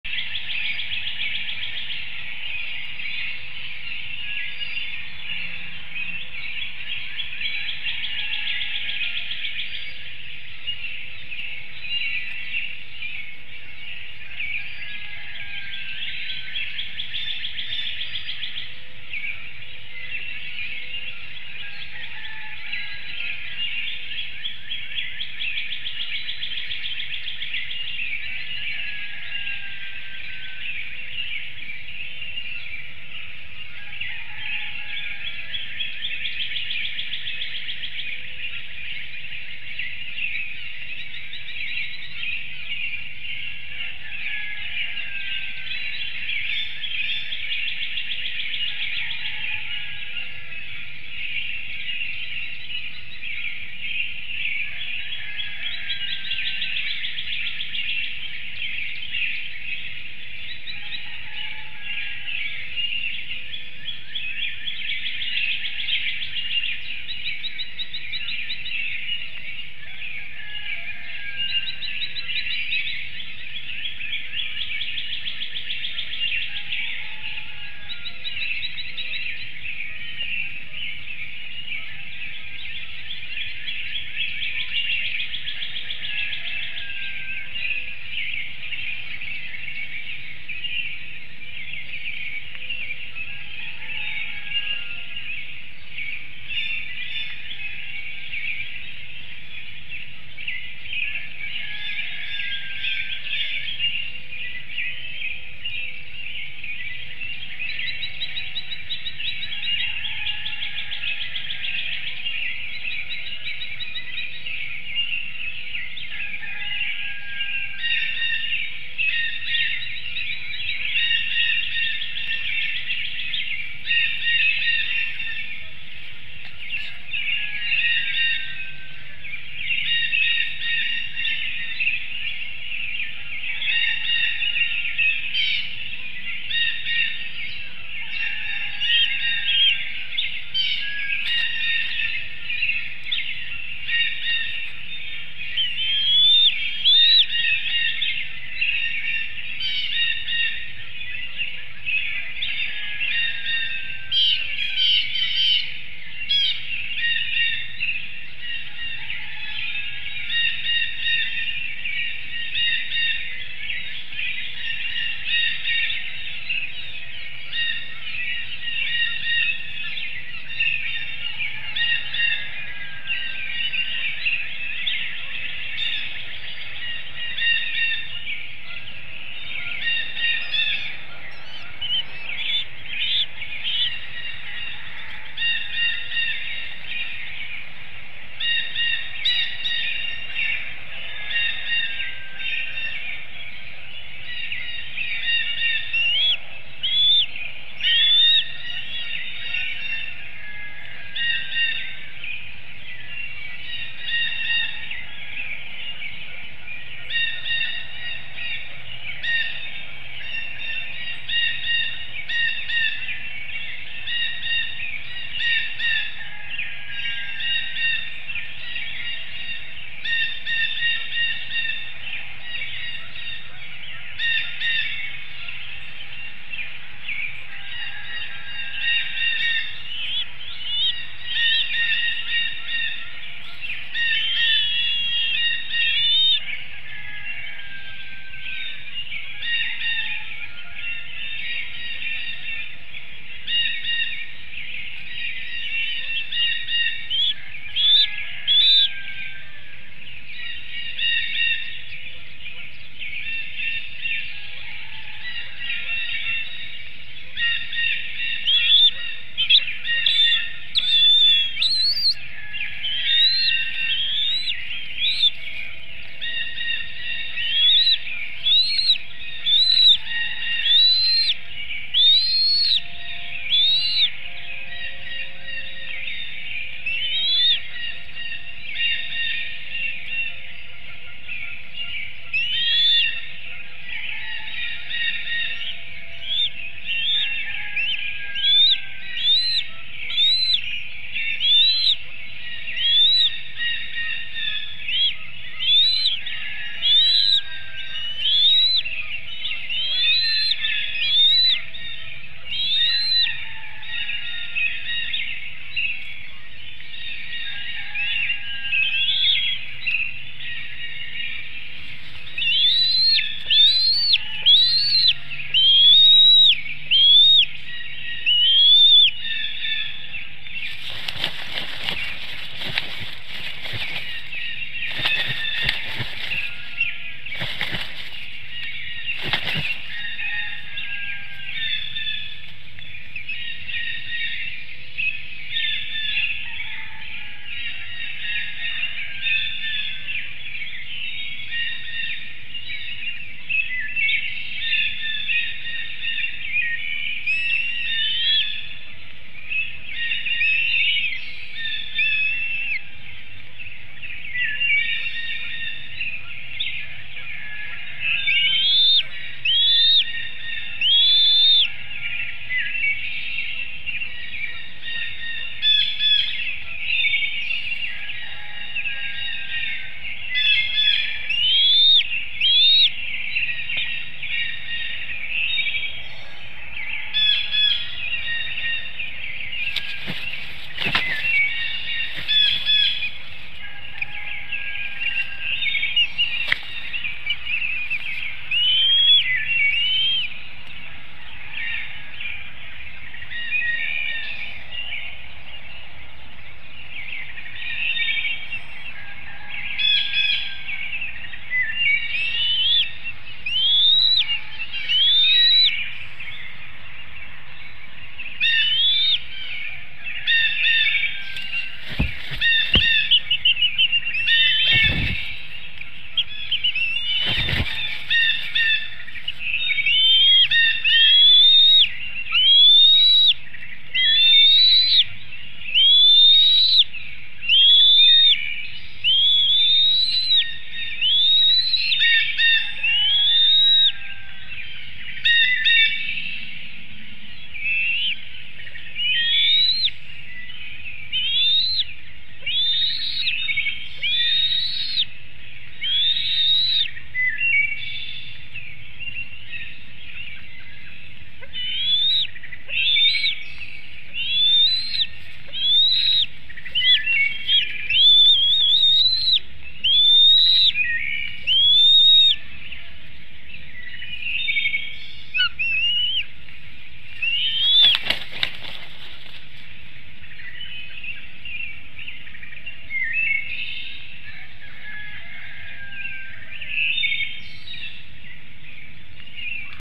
Liberty finishes nightshift at the Webcam in Hanover, PA | HDonTap | 05-01-19 6:05 AM
In the early morning on 05-02-19, Liberty rises and stretches by flapping her wings.  At 6:05 a.m. EDT, she flies out to the sentinel tree for a break after overnight duties at the Hanover, PA nest.

Being that there's no food at the nest, either Liberty or Freedom needs to hunt so breakfast can be fed to Stars and Stripes.